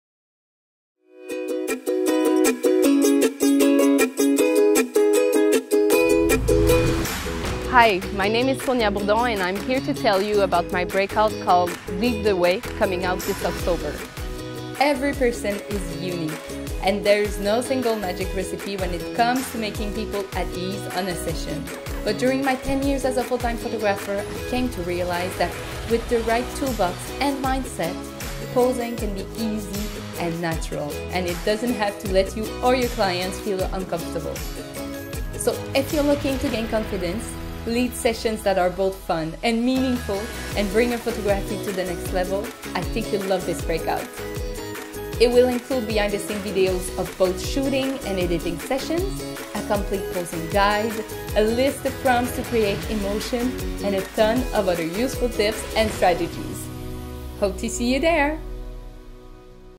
Lead The Way with Sonia Bourdon
Sonia Bourdon’s approach to photographing people is mostly inspired by connection and light. She’s committed to creating an amazing experience for her clients, from the first email to the images delivery.

In this Breakout, she’ll share with you how she finds the best location scenarios for creating beautifully lit images. You’ll get to see how she prepares herself and her clients before a session to strengthen their trust, and ease the whole process of posing during the session.

She’ll give you posing tips to create genuine connection between your subjects and take you behind the scene to discover how she leads a session and interacts with her clients to create authentic images. She’ll also share with you, along with examples, how she overcomes some difficult situations that might arise during a session. You’ll also get to see her complete workflow, and how she delivers the final product.